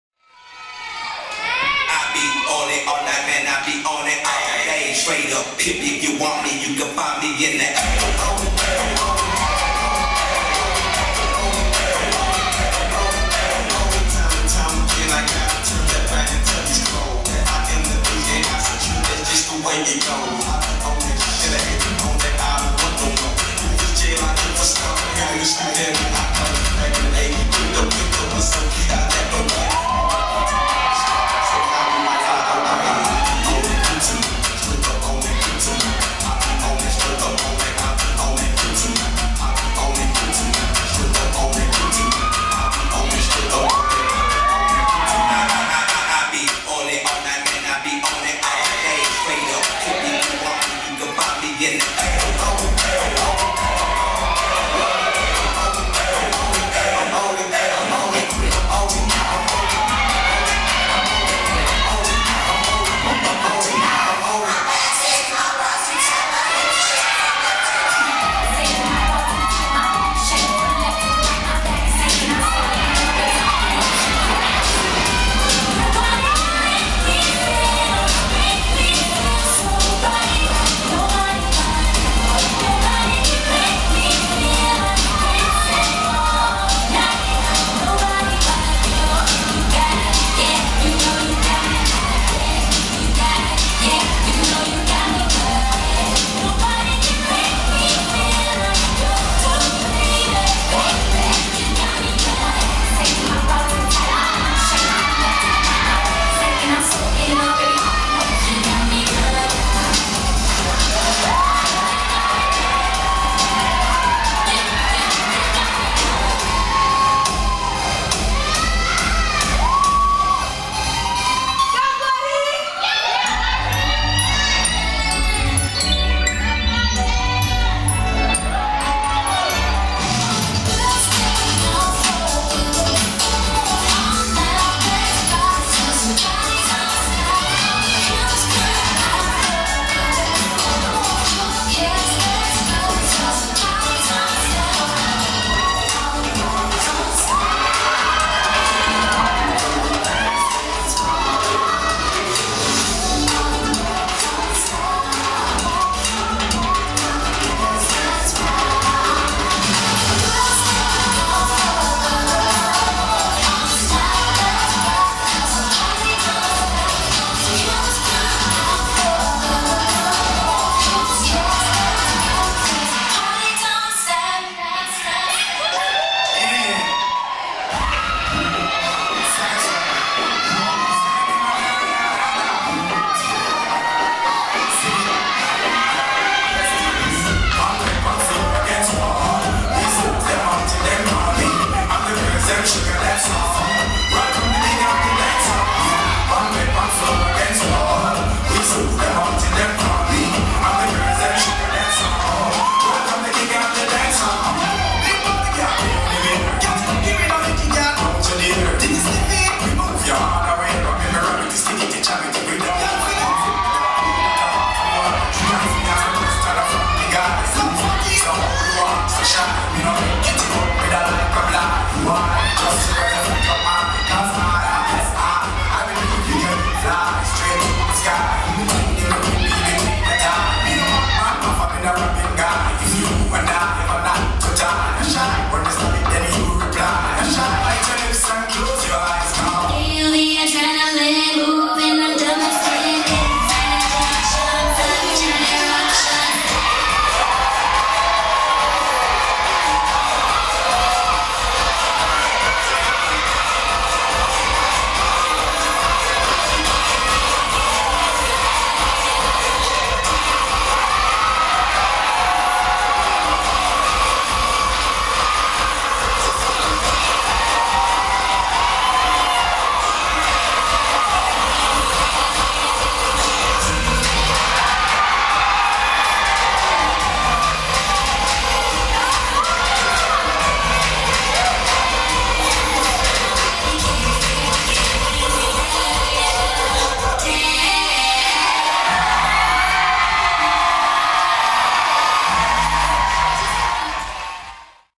BDCP Adults at I Can B'More 2013 [OFFICIAL]
BDCP Adults perform at the first annual I Can B'More Urban Dance Showcase.

Notre Dame of Maryland University, LeClerc Hall